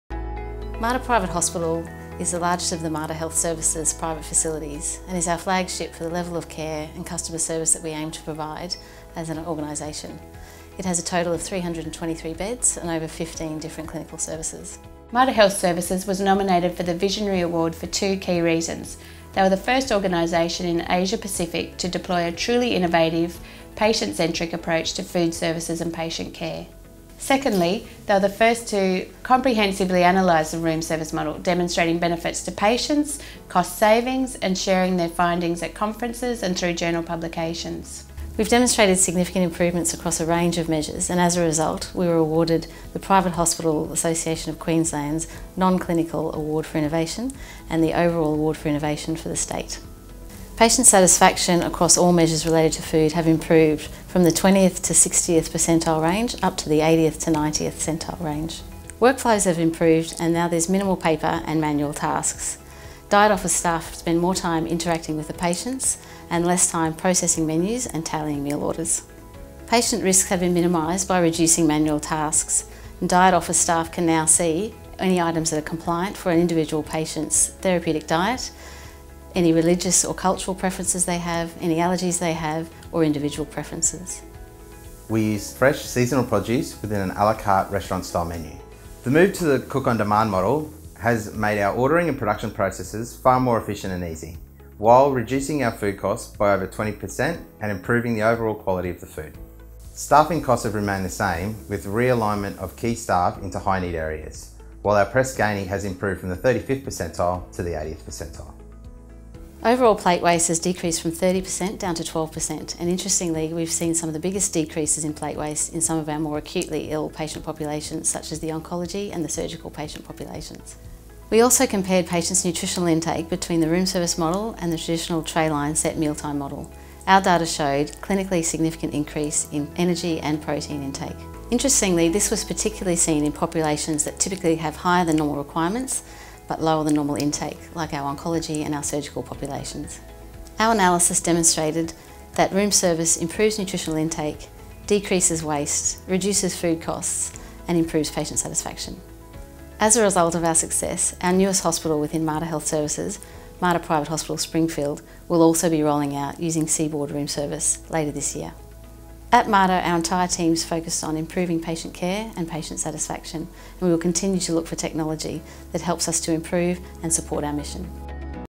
Mater Private Hospital Brisbane
Putting patients first requires more than world-class clinical care, so Mater Private Hospital Brisbane had the idea to take quantifiable data to demonstrate how Room Service Choice® would escalate patient experience and nutritional intake, while minimizing cost, plate waste, and patient risk.